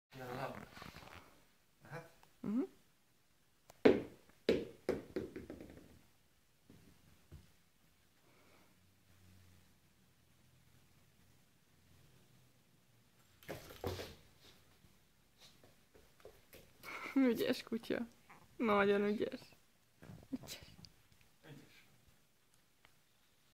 Golden Retriever Mira and the ball
Mira pounces and catches her toy ball